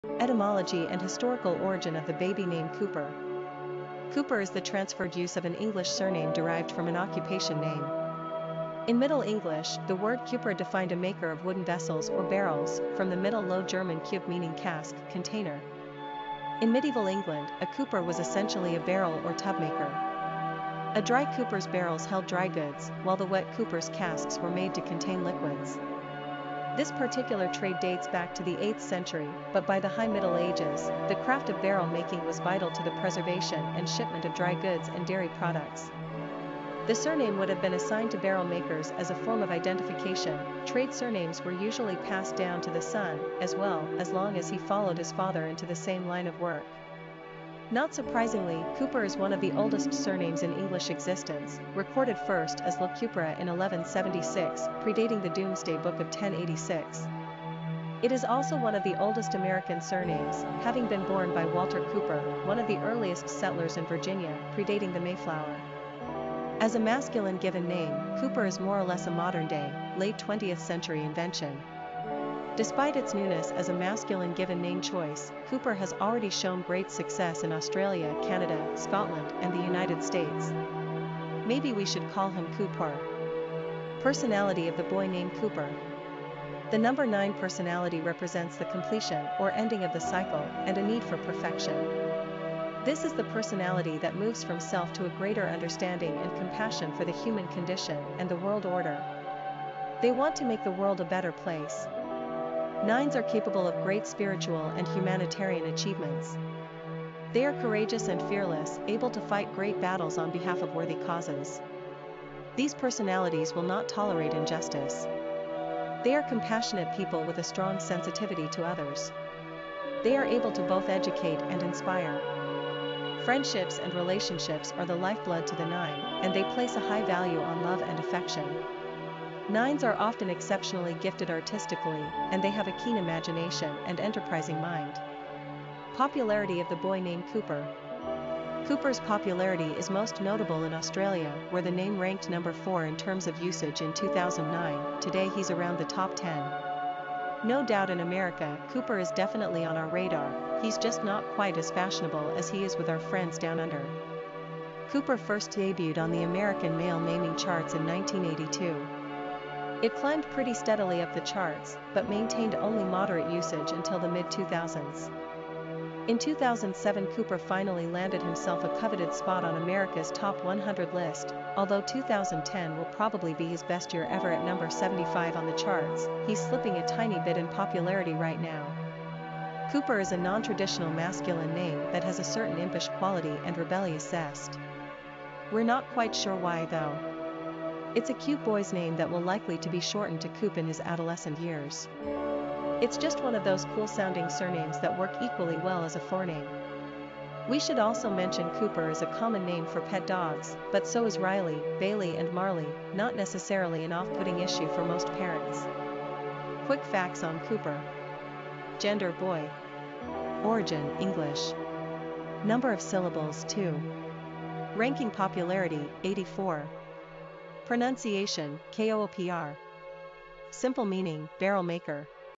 Cooper - Boy Baby Name Meaning, Origin and Popularity
Cooper is the transferred use of an English surname derived from an occupation name.